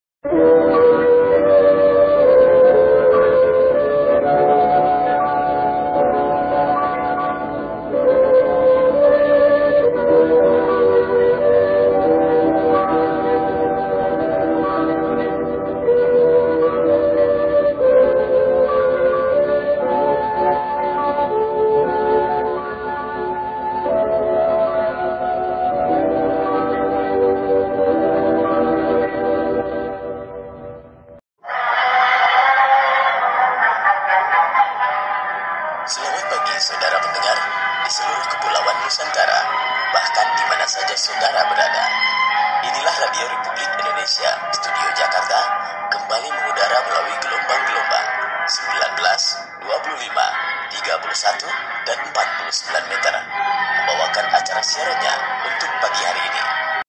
Suara Dachri Oskandar Penyiar RRI || Sangat familiar bagi Generasi OLD
Mendengarkan suara pembuka siaran Radio Republik Indonesia (RRI) ini  mengingatkan kembali para Generasi OLD ke masa lalu.
Jaman dulu hiburan di rumah, khususnya masyarakat pas pasan hanya berupa radio transistor. Dan siaran radio lokal gelombang MW yang tersedia hanya dari stasiun RRI.
Tetapi jika ingin dengar berita dari luar negeri bisa setel di gelombang SW, maka akan menangkap siaran radio luar negeri.

Orang-2 jaman itu untuk update berita, setiap hari menyetel radio RRI dan pasti yang keluar pertama adalah suara seperti di video ini.